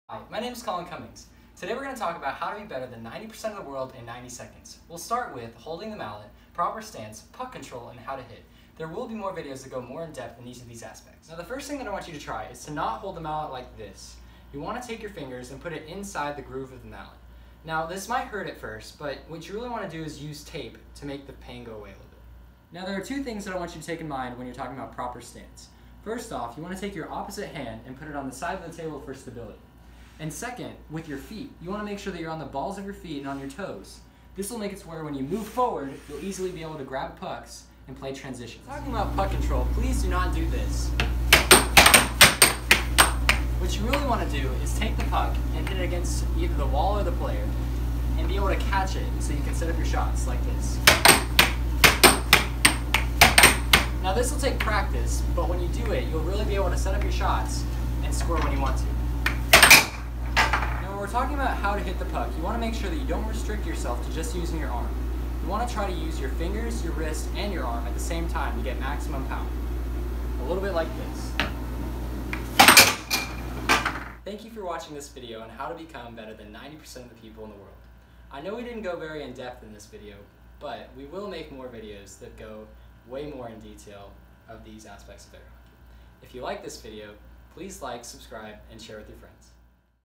How to Beat 90% of the World in Ninety Seconds! (in air hockey)
Ever wanted to beat your best friend at air hockey? In this video, we will explore a few basic techniques that will help you become a better air hockey player in just ninety seconds!